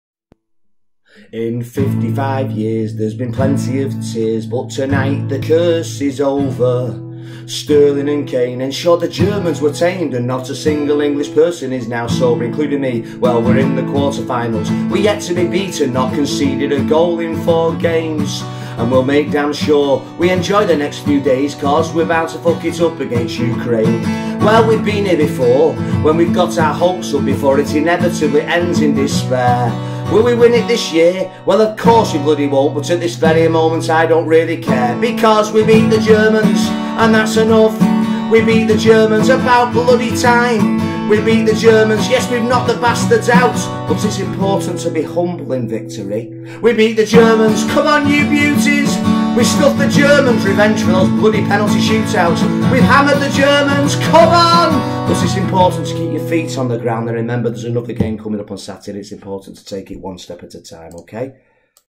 We Beat The Germans - Steve Canavan
After decades of disappointments we've actually done it - beaten the bloody Germans at football. It'll never happen again, obviously, so let's make the most of it. Euro 2020.